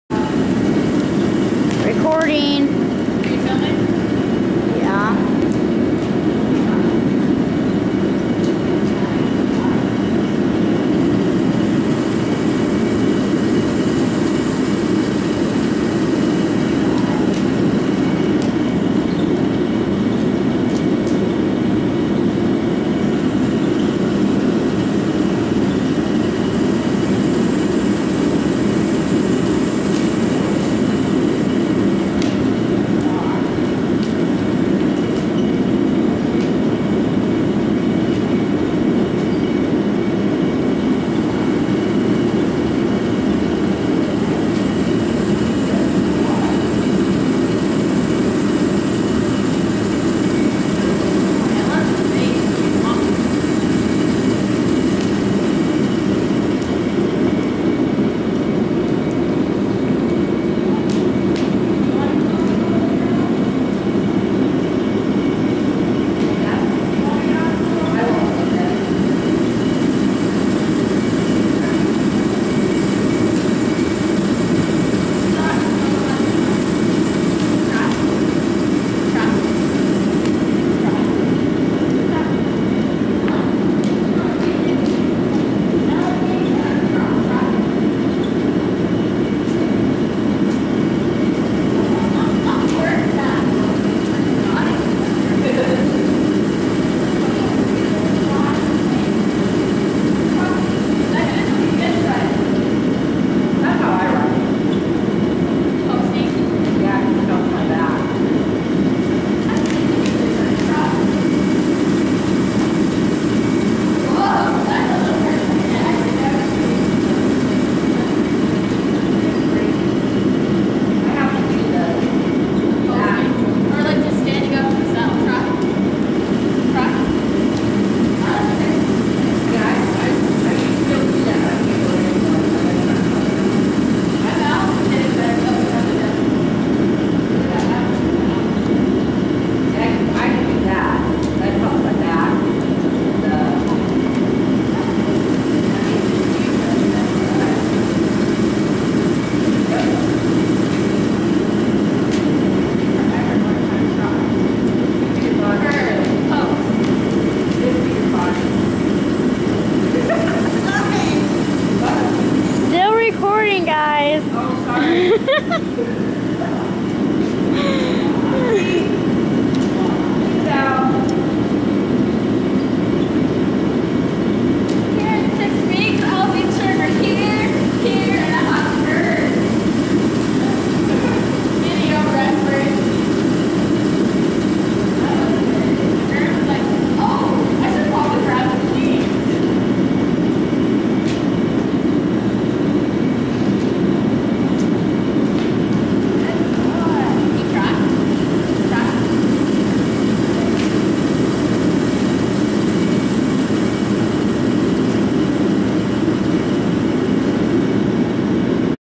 Me trotting Spark 11-3-13
Me trotting Spark in the indoor arena :)